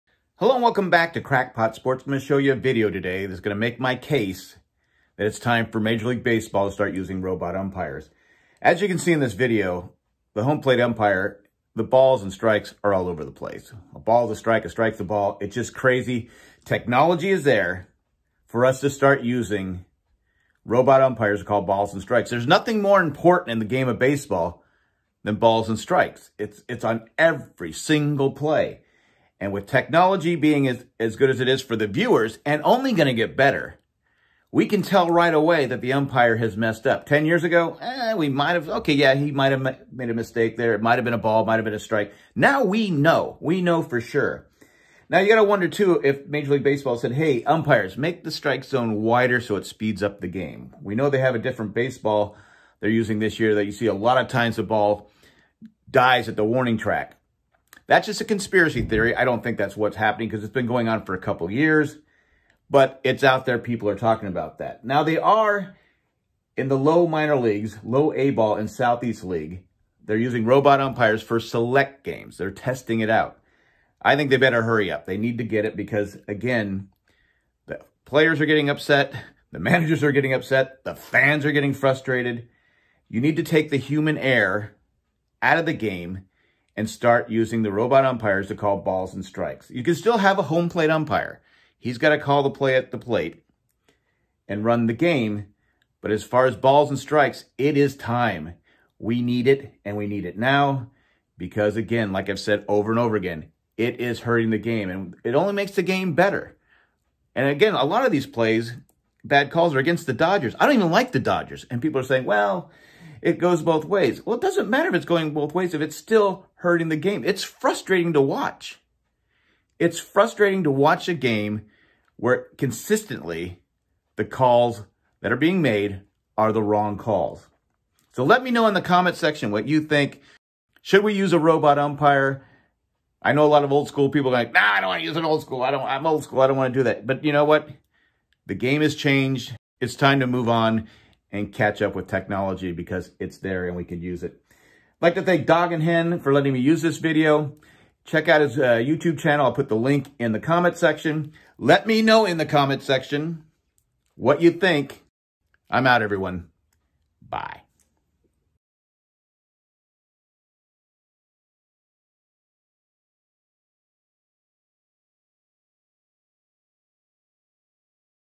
It's Time for Robot Umpires
It's time to use Robot Umpires to Call Balls and Strikes
Special Thanks to Dawg&Hen for the use of his video. Here is the link to his youtube channel, make sure you check it out